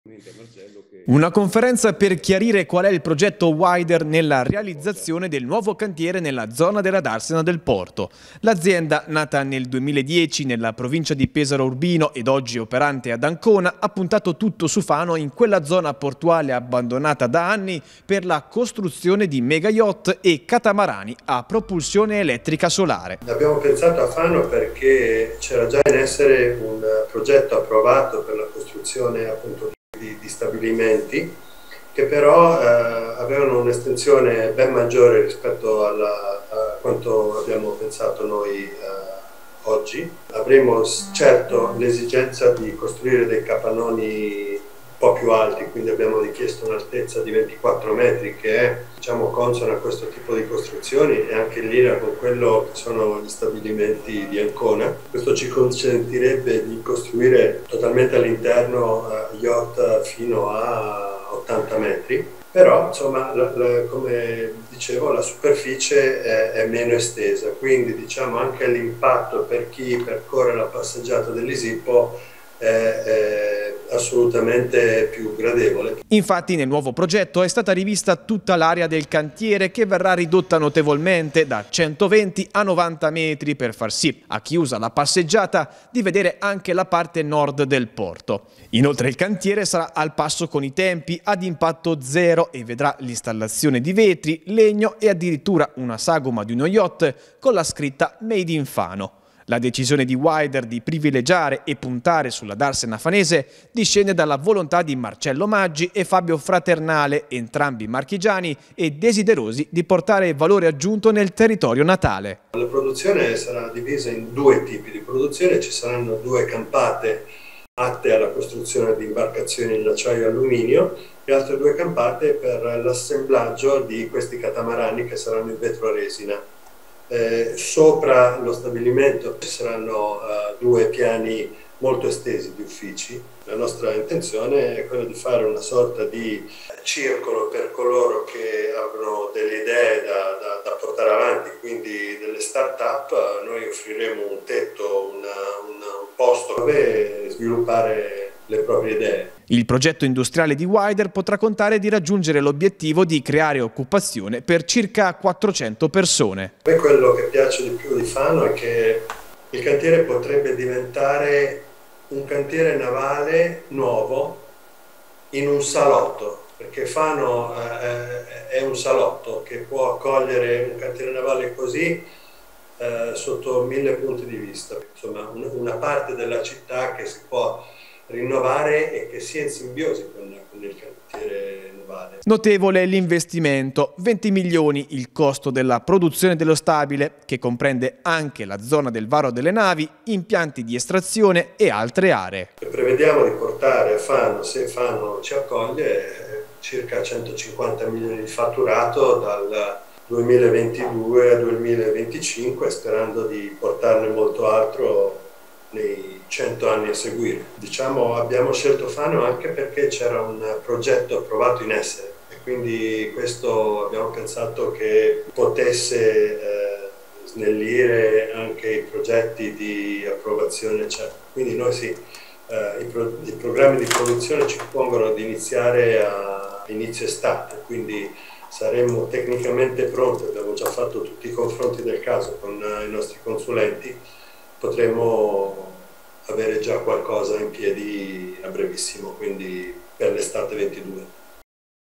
Porto Fano, ecco la proposta del nuovo cantiere Wider
Ecco la proposta per il cantiere navale a Fano: nella struttura di legno e vetrate nasceranno gli superyacht del futuro. Il progetto prevede 400 nuovi posti di lavoro e accordi con le università e i centri di ricerca.